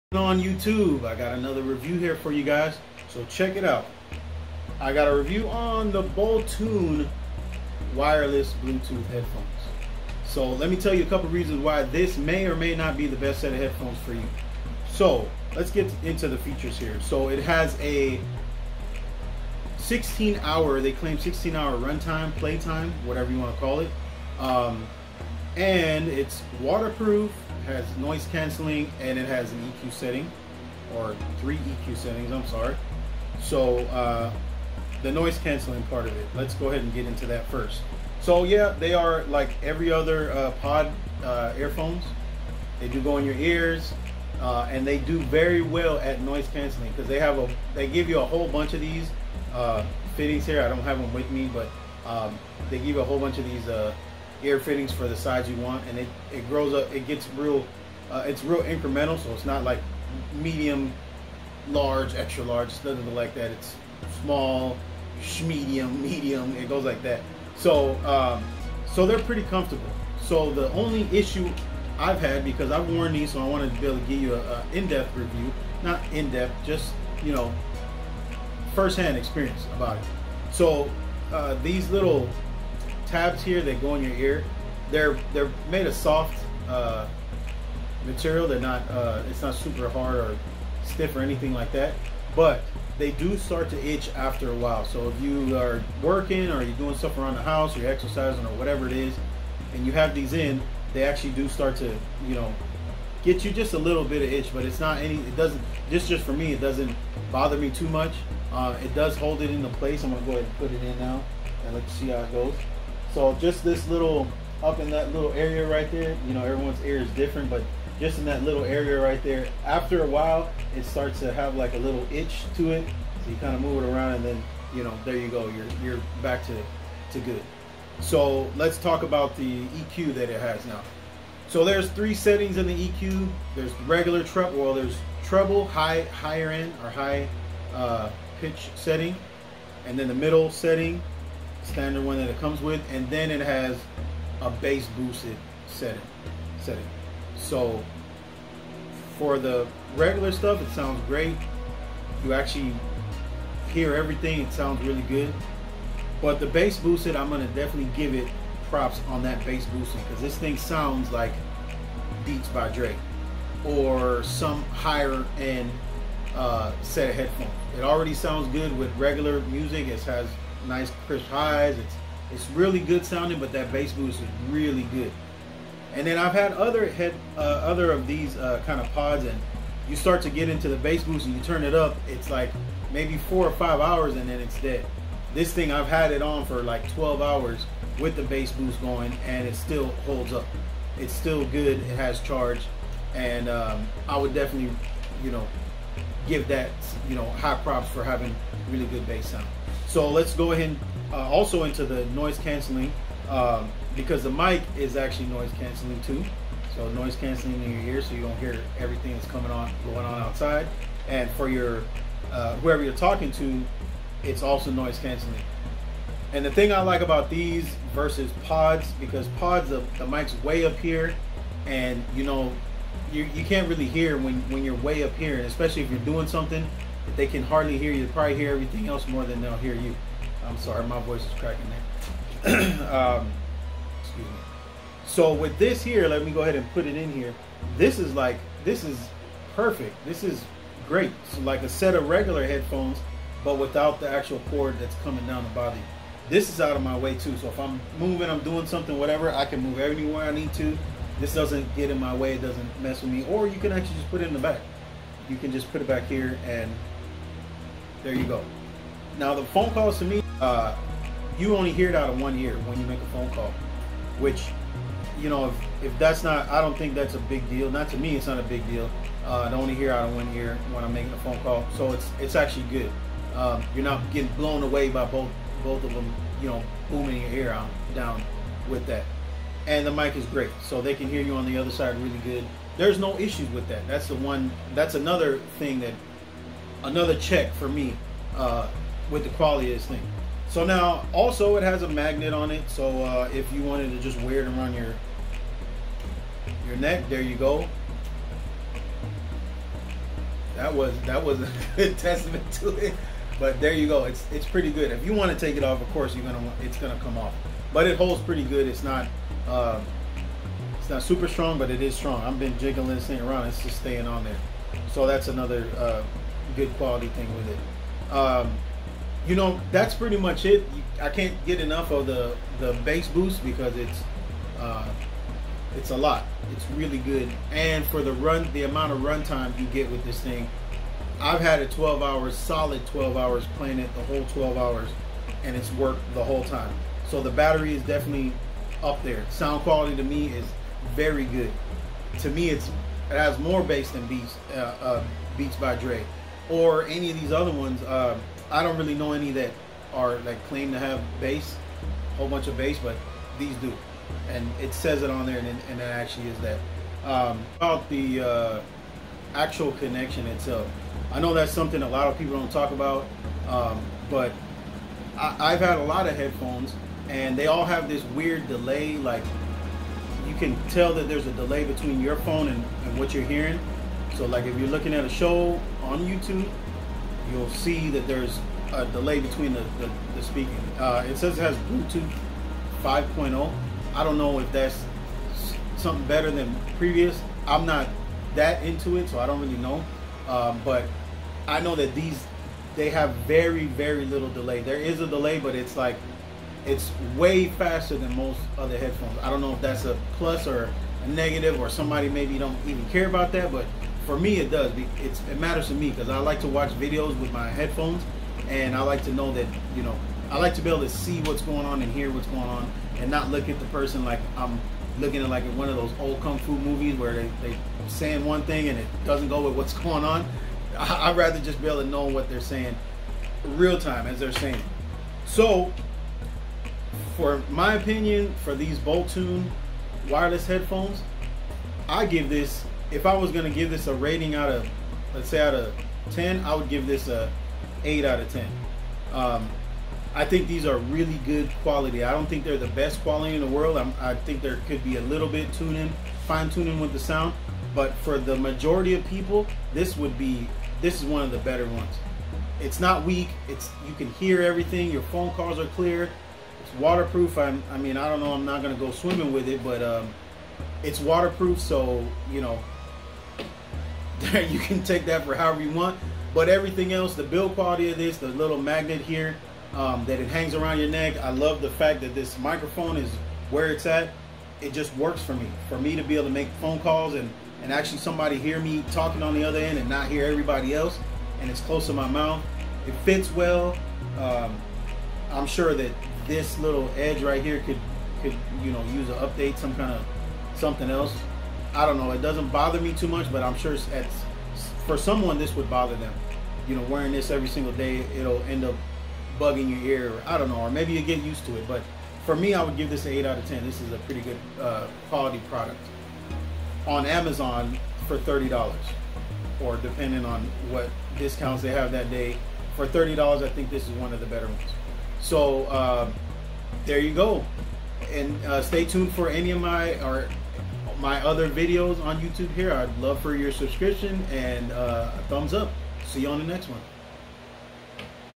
Boltune bluetooth wireless headphones
This is a review of the BOLTUNE bluetooth Wireless headphones. This can be found on amazon. Enjoy.